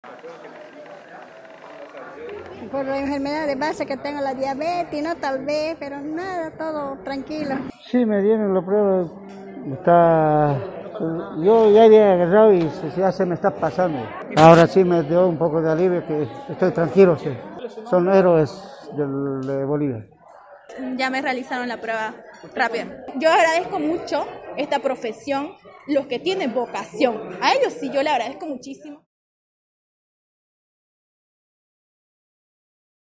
Comerciantes del “2 de Agosto” agradecen labor de brigadistas
Comerciantes del mercado 2 de Agosto agradecen la labor de las brigadas médicas que visitaron sus puestos de trabajo para darles atención y medicamentos, además se realizaron las pruebas rápidas de manera gratuita a los que presentaron algunos síntomas del Covid-19.